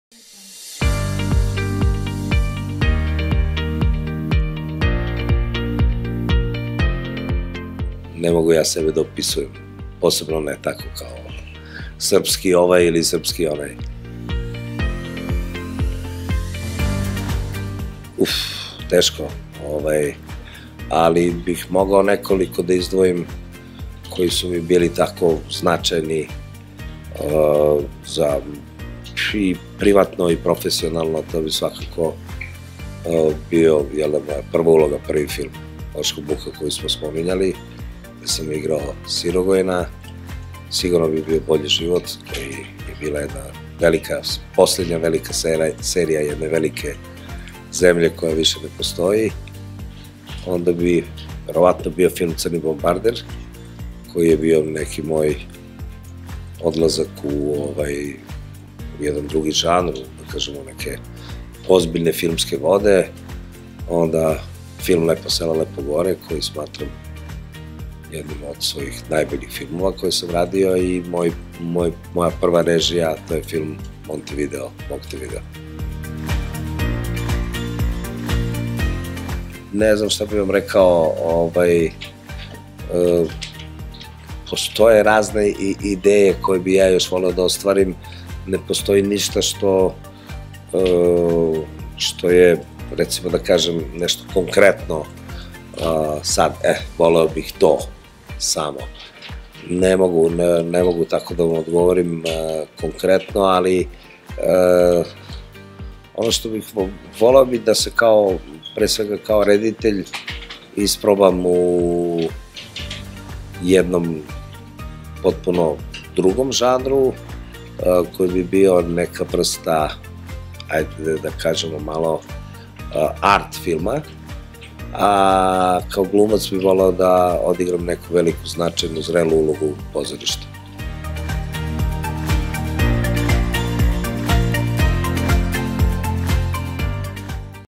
28. SOF intervju - Dragan Bjelogrlić
Na Vročo SOFo je sedel eden največjih zvezdnikov v regiji, srbski Robert Redford ‒ igralec, producent, scenarist in režiser Dragan Bjelogrlić – Bjela.